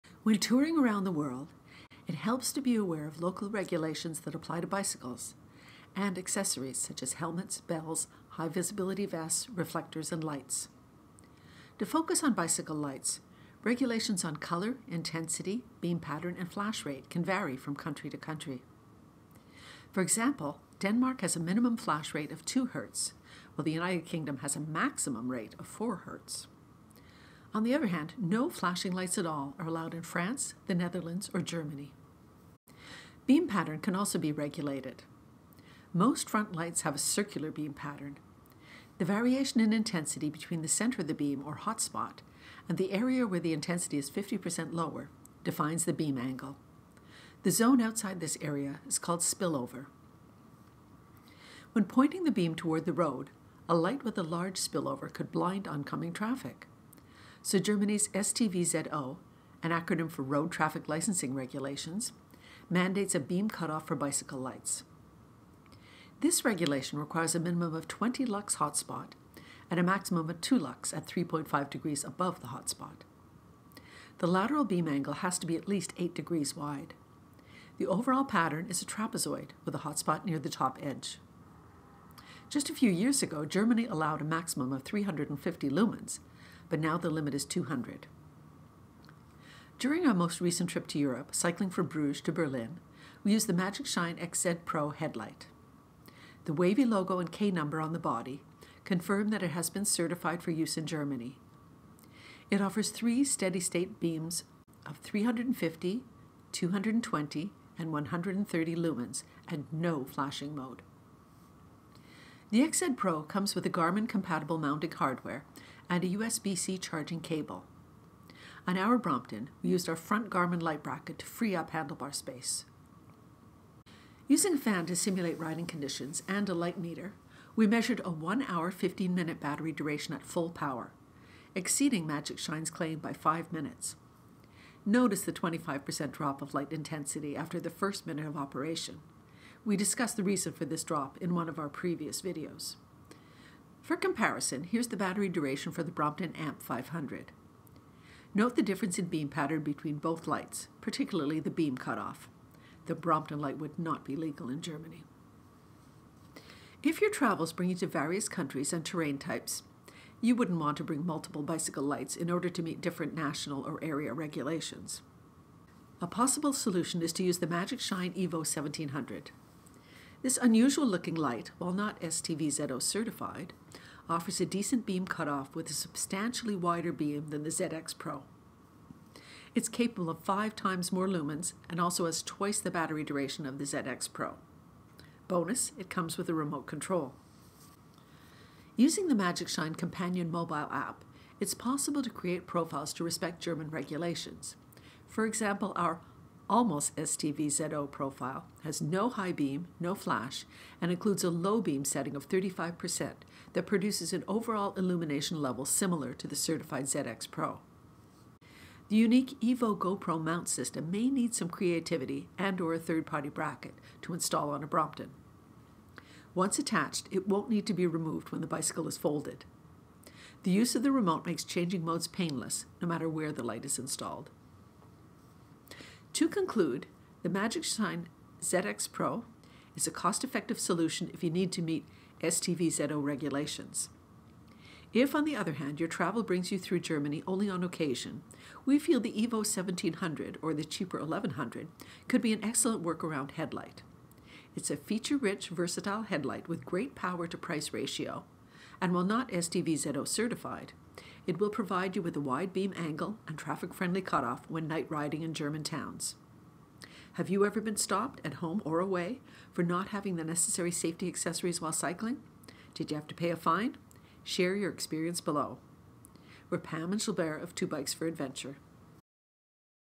Bicycle Headlight regulations [ Magicshine ZX Pro vs EVO 1700 ]
When touring around the world, it helps to be aware of local regulations.
Germany StVZO mandates maximum intensity and beam cutoff for all bicycle headlights. Let's compare the Magicshine XZ Pro and EVO 1700 lights and see the pros and cons of each.

00:00 Introduction
00:19 Flash Rate Regulations
00:35 Beam Patterns
00:57 StVZO beam requirements
01:33 Magicshine ZX Pro StVZO
02:36 Magicshine EVO 1700
03:46 Conclusion

Copyrights
Policeman OpenClipart-Vectors from Pixabay